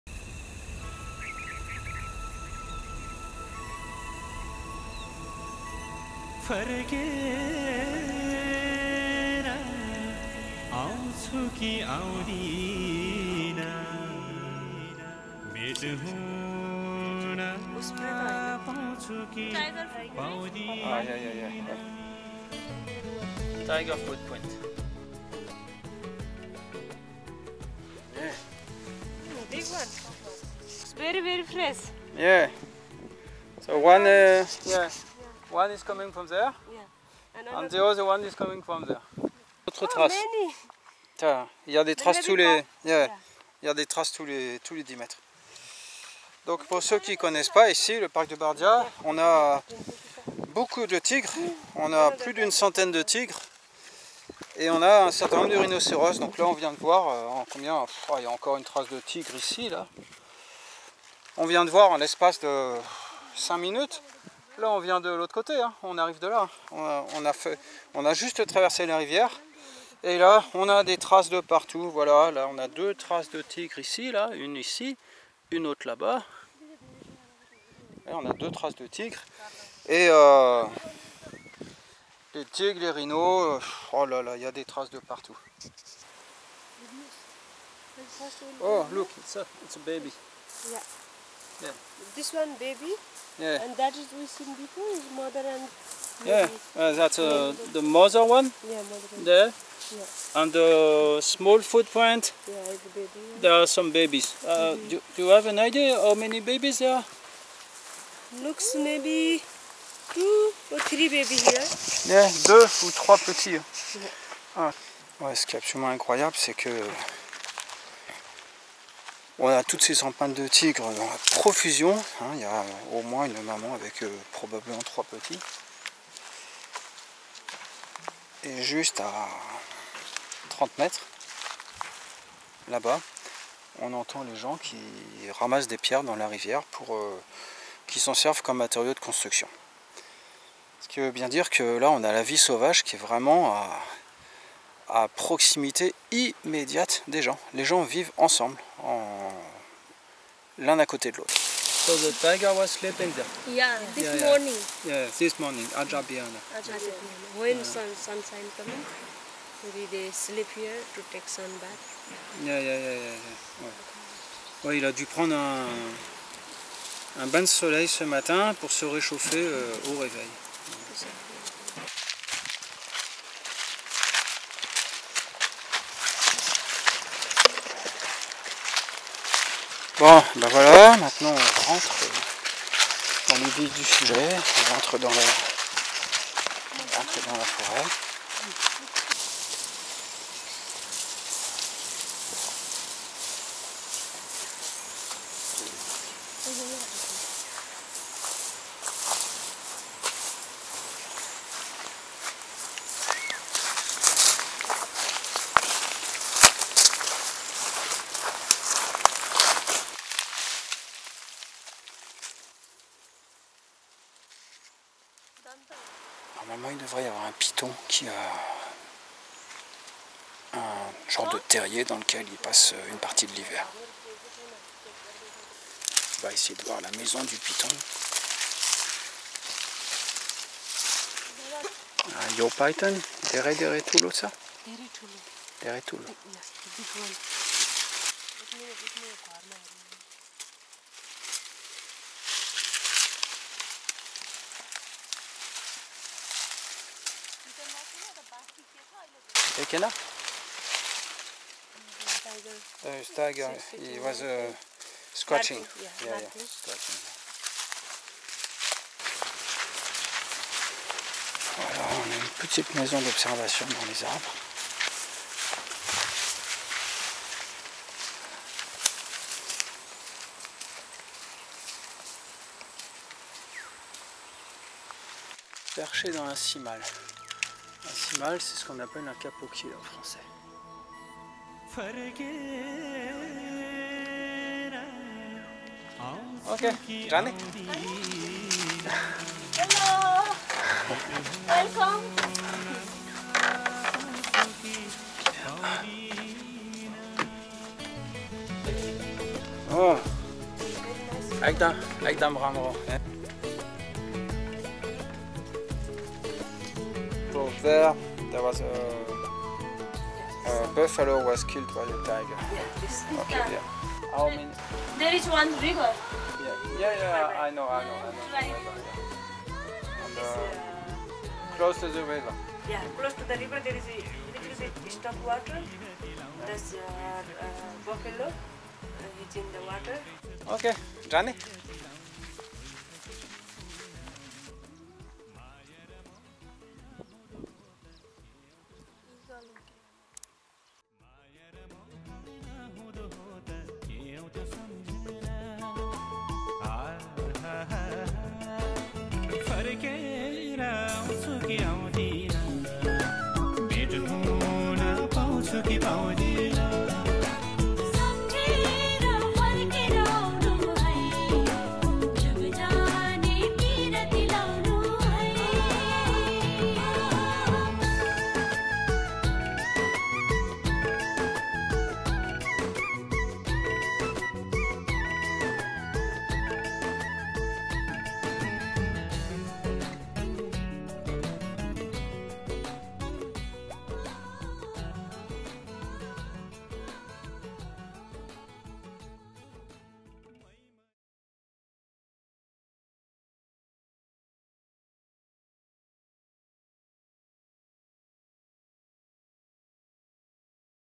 Sur la piste du tigre avec Manju et Susila
Partez à la recherche du tigre du Bengale avec deux jeunes guides naturalistes dans le parc national de Bardia au Népal. Une expérience au plus près d'une nature sauvage.